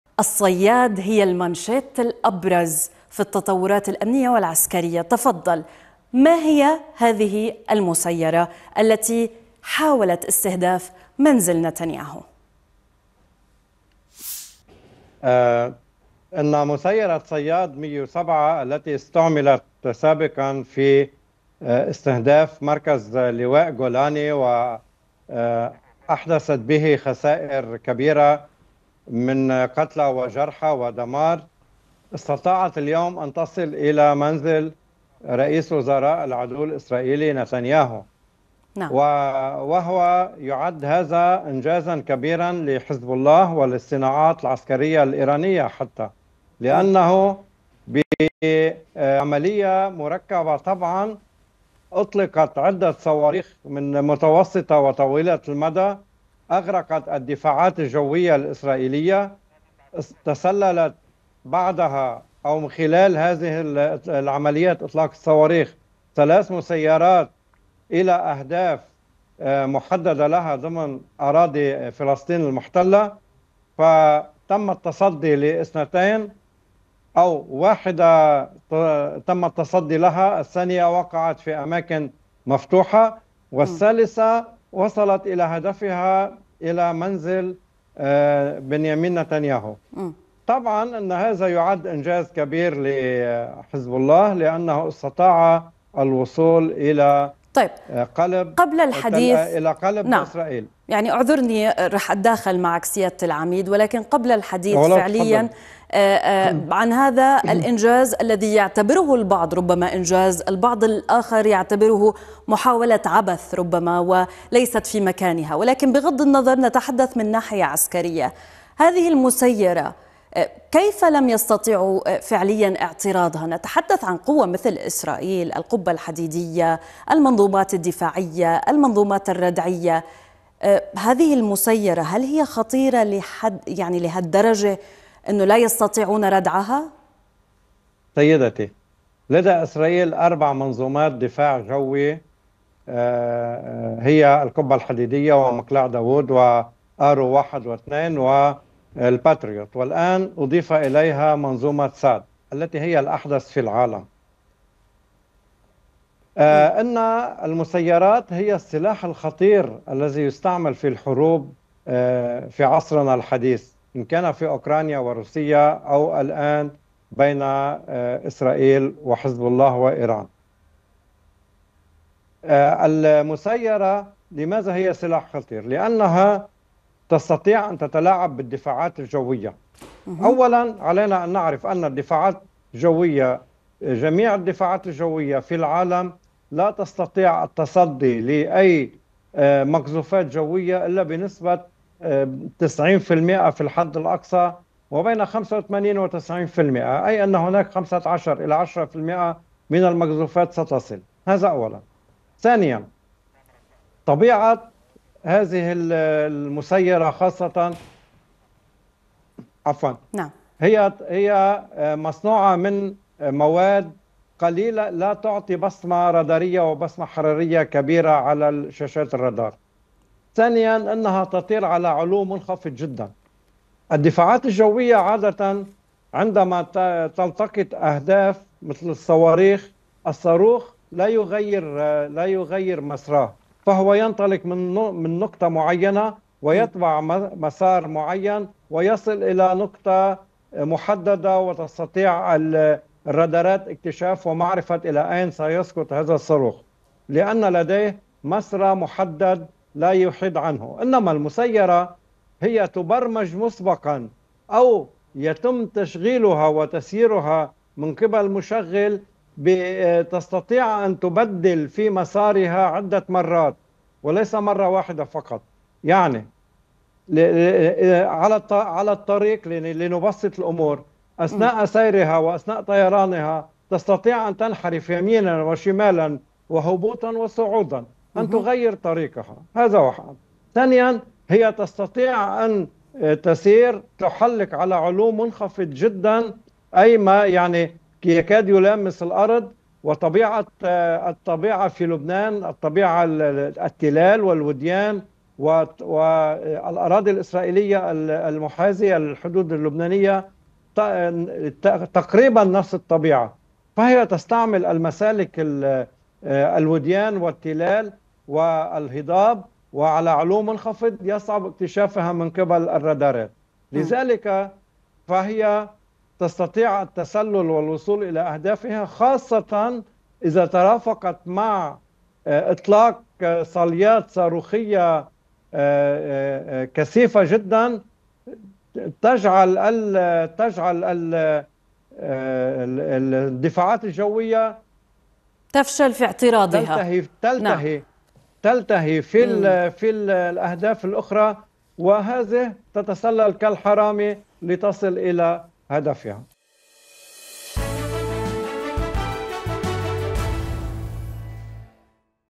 كل ما تود معرفته عن مسيرة "صياد 107" الايرانية التي استهدفت منزل نتنياهو
موقع قناة التغيير الفضائية  الالكتروني